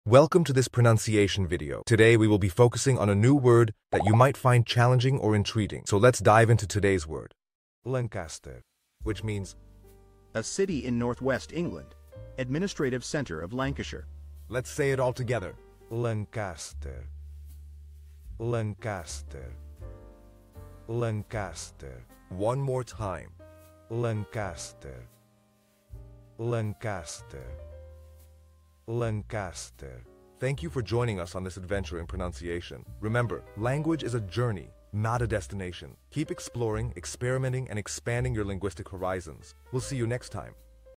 How to pronounce Lancaster
Master the Pronunciation of 'Lancaster WHICH MEANS LANCASTER' - which means : City in Northwest England;ADMINISTRATIVE CENTER OF LANCASHIRE. 📝 with @PortuguesePronunciation 🎓🗣️ - [Your Guide to Portuguese]

Learn to pronounce 'Lancaster' perfectly in Portuguese with our easy and engaging tutorial by @PortuguesePronunciation! 📖📽️ In this comprehensive video, we provide you with native speaker examples 🗣️👂 and phonetic breakdowns 🎵 to ensure you get the pronunciation just right.

How to say Lancaster in Portuguese